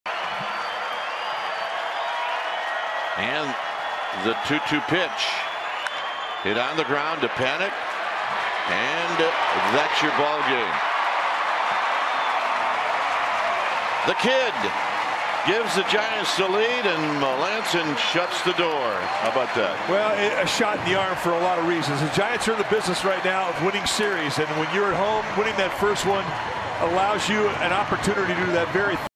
SD@SF: Melancon induces a groundout to end the game
Mark Melancon gets Cory Spangenberg to ground out to end the game, notching the save and securing the 4-3 win over the Padres